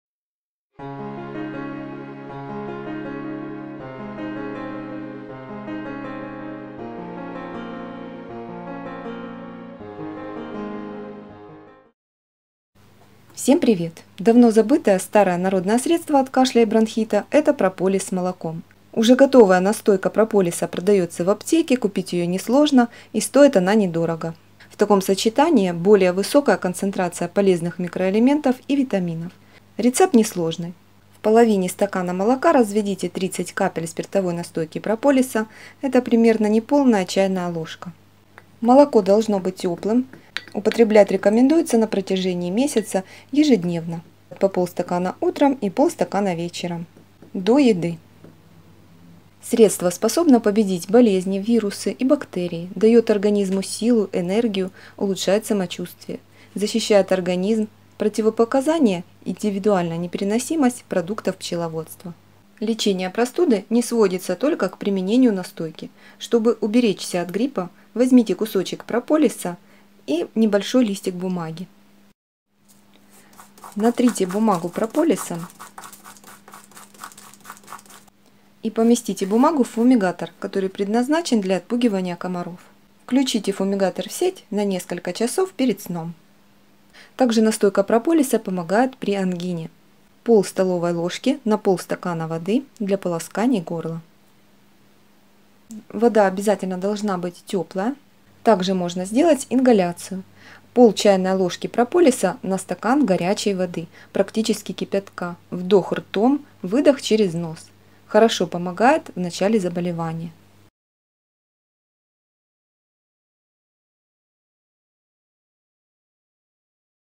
Фантастика а не средство. Кашель, бронхит
Старое, забытое, но эффективное и рабочее народное средство от кашля, бронхита и ангины.